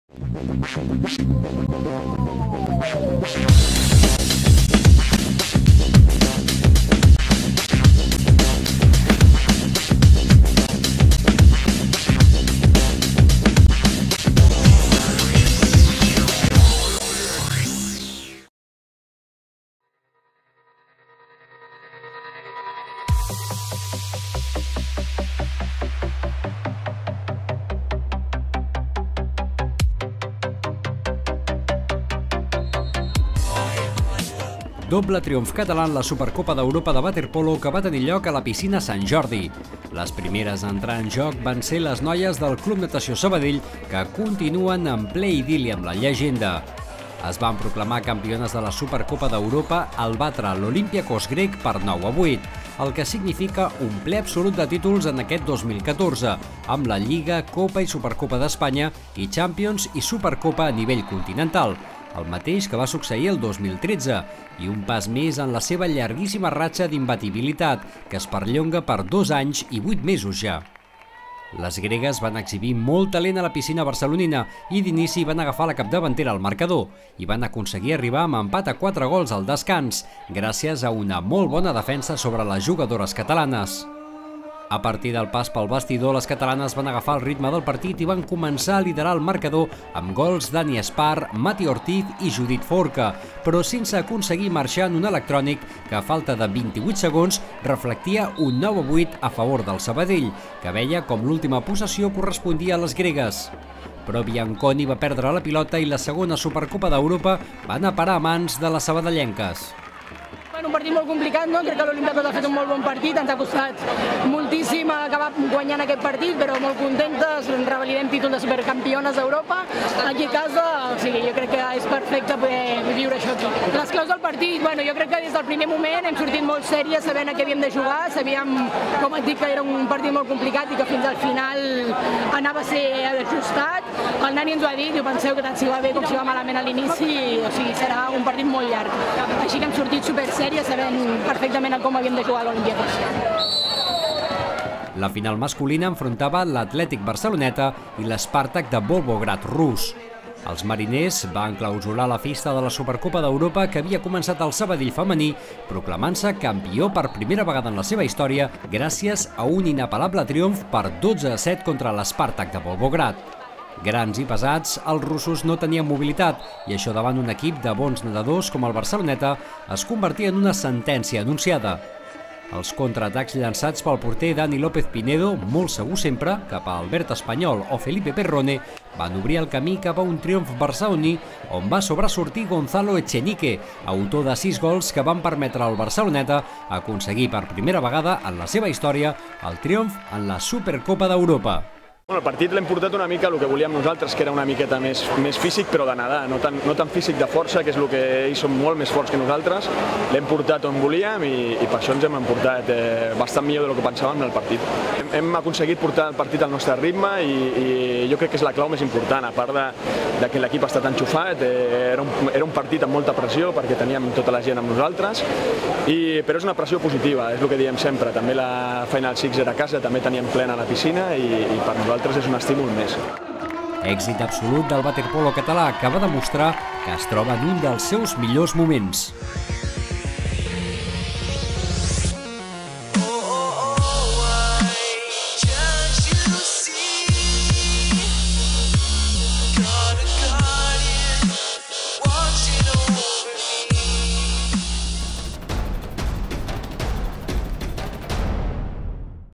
Supercopa d'Europa de waterpolo
Club Natació Sabadell i Atlètic Barceloneta es van alçar amb la Supercopa d'Europa disputada a la piscina Sant Jordi.